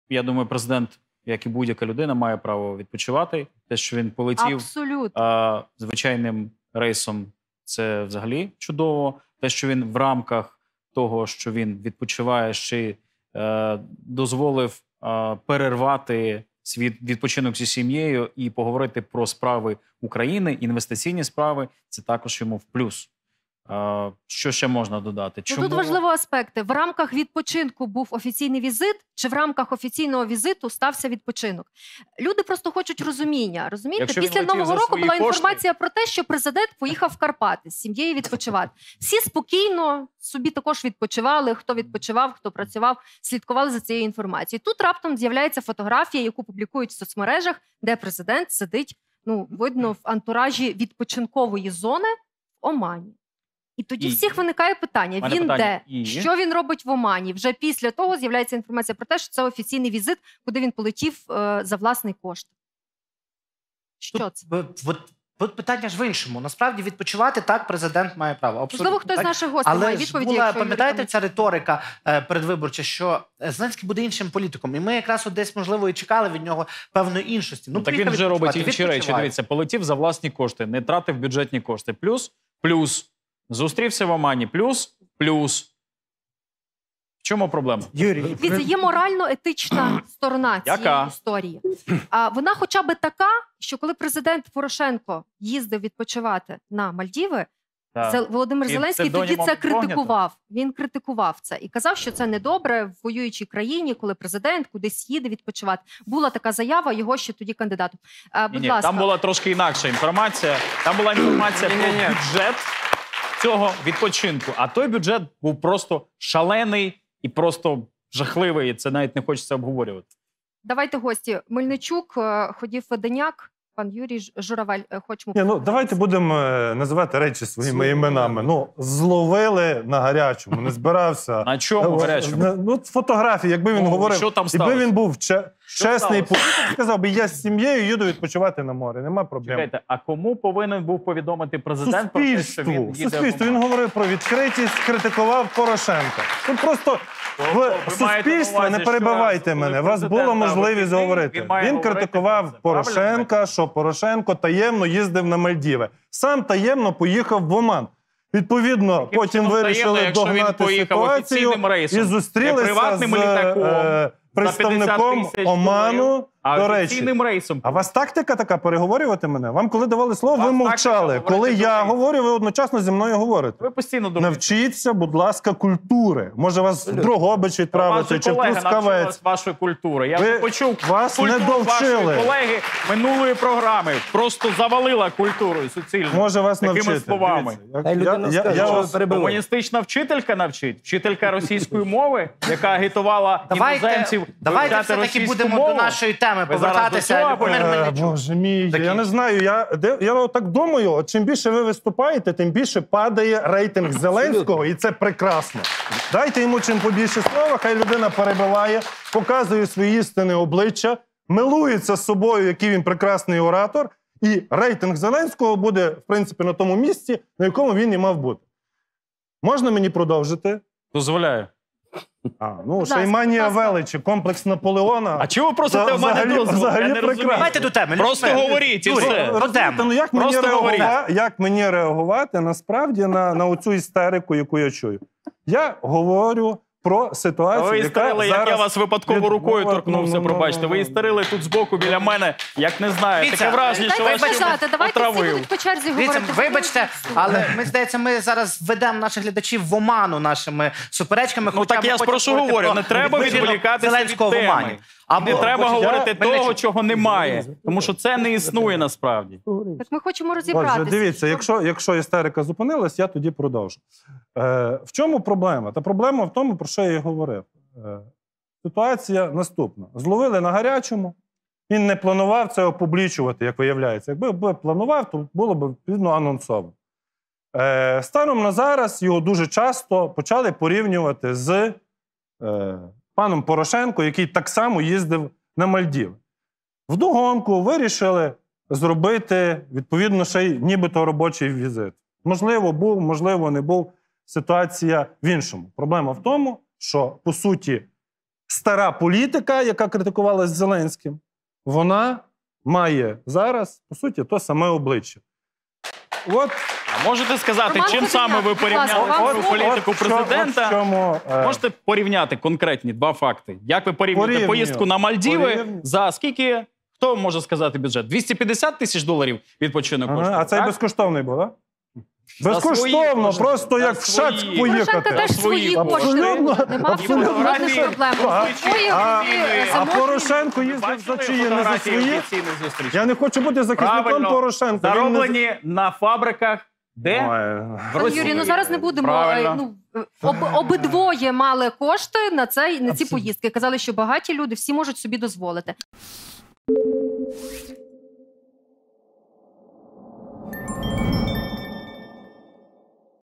💥Cвободівець Любомир Мельничук ПОСТАВИВ НА МІСЦЕ "слугу народу" Юрія Камельчука
💥"ПРИПИНІТЬ ІСТЕРИТИ❗" - свободівець Любомир Мельничук ПОСТАВИВ НА МІСЦЕ "слугу народу" Юрія Камельчука. Через "візит" Зеленського в Оман РОЗГОРІЛАСЯ ЗАПЕКЛА СУПЕРЕЧКА❗ ХТО, НА ВАШУ ДУМКУ, МАЄ РАЦІЮ ПИШІТЬ У КОМЕНТАРЯХ!